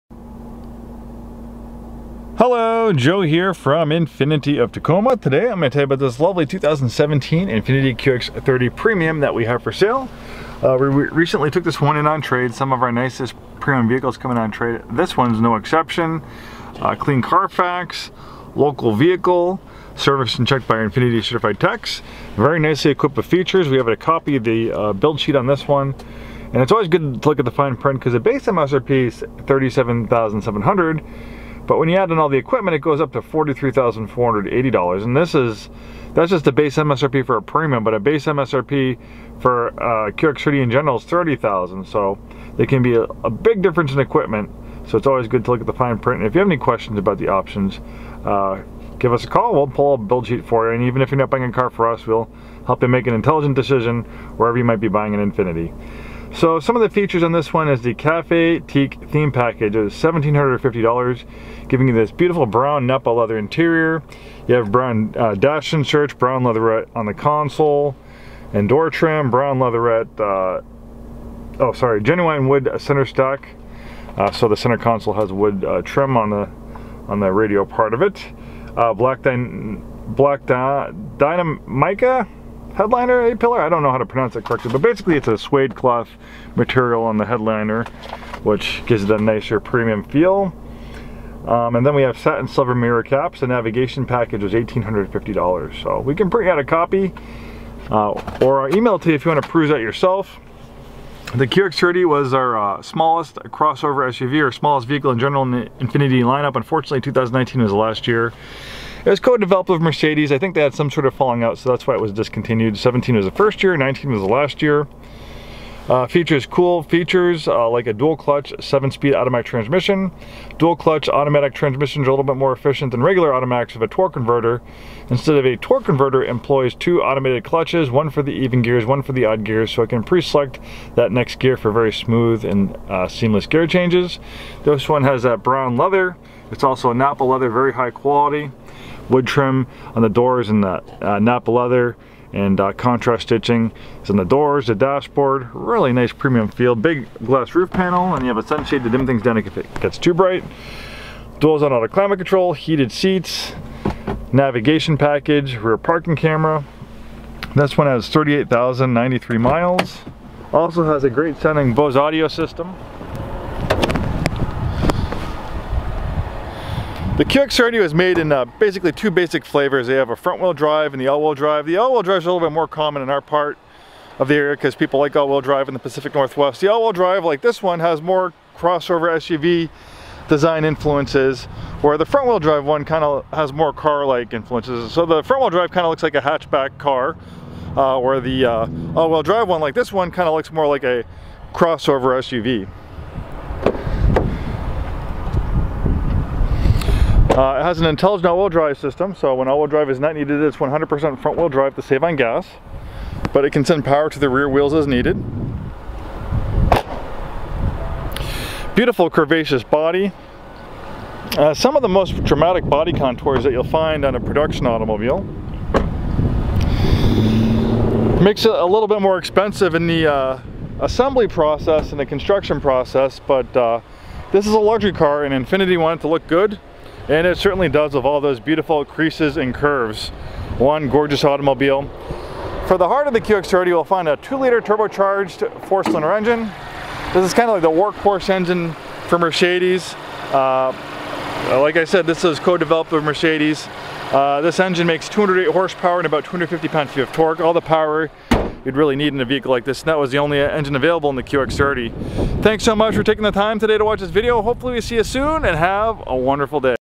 2017 INFINITI QX30 Premium AWD With Cafe Teak Theme Package
The QX30 is the smallest crossover in the INFINITI line, but dont let its size fool you, it has a surprisingly spacious interior for its size. This is the AWD model which offers more ground clearance and more crossover SUV styling versus the more car-oriented front wheel drive version. A 7-speed dual clutch automatic transmission pairs very nicely to a powerful yet fuel efficient direct injection turbo four engine making nearly 210-hp.